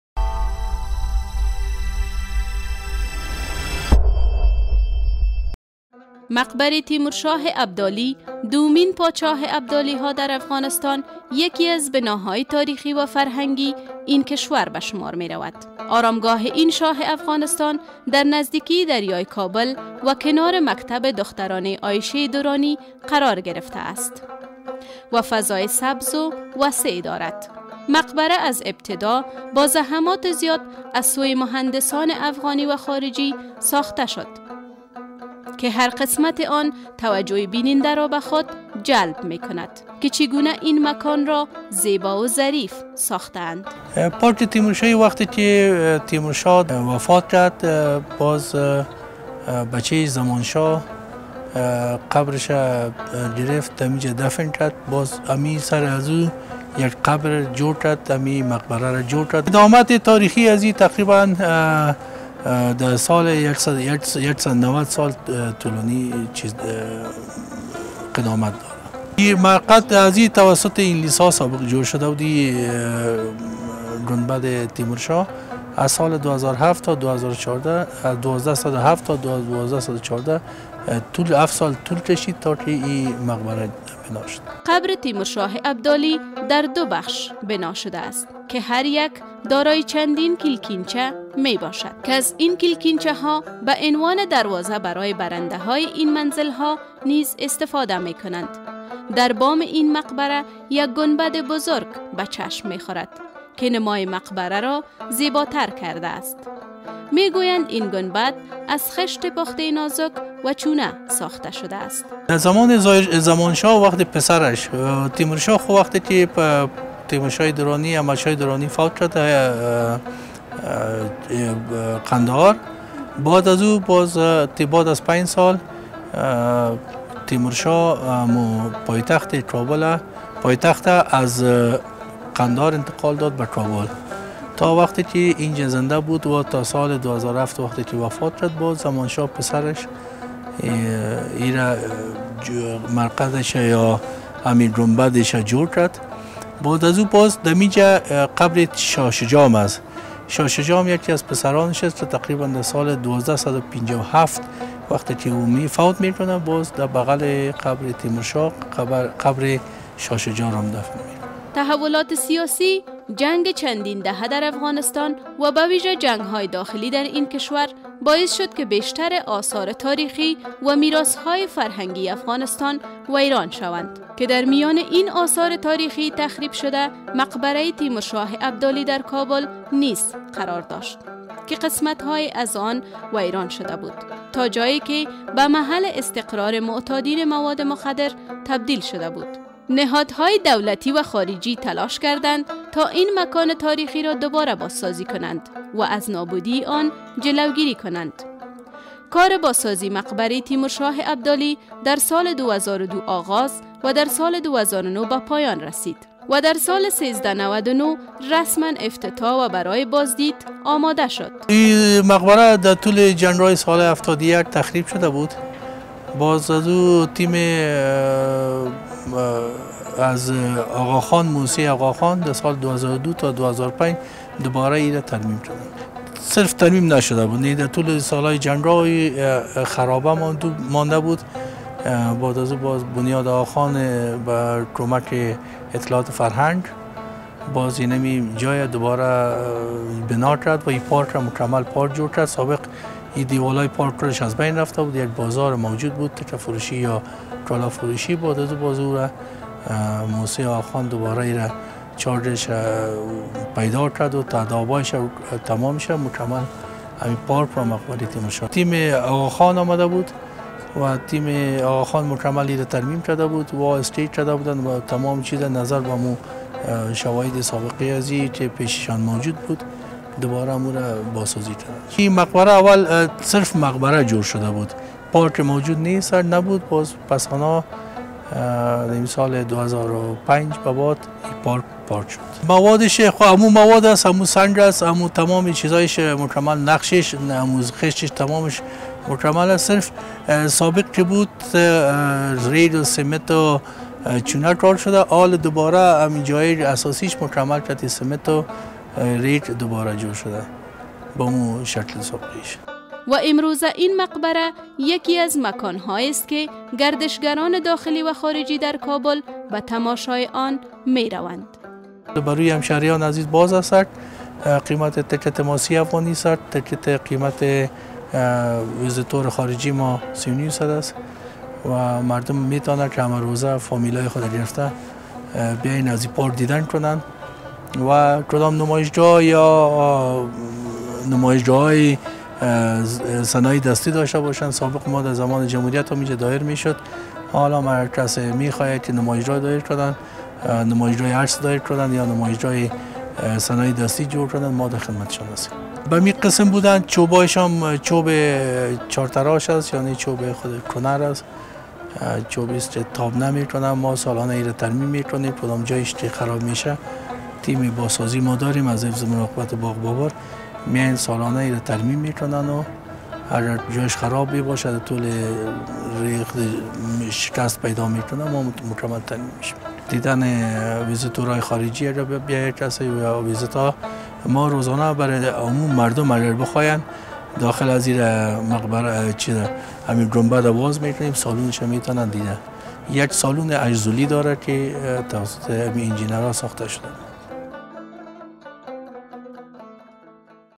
تیمور شاهی کابل
مقبره تیمور شاه ابدالی دومین پادشاه ابدالی ها در افغانستان یکی از بنا های تاریخی و فرهنگی افغانستان است، آرامگاه این شاه افغانستان در نزدیکی دریای کابل و کنار مکتب دخترانه ی عایشه درانی قرار گرفته است و فضای سبز و وسیع دارد.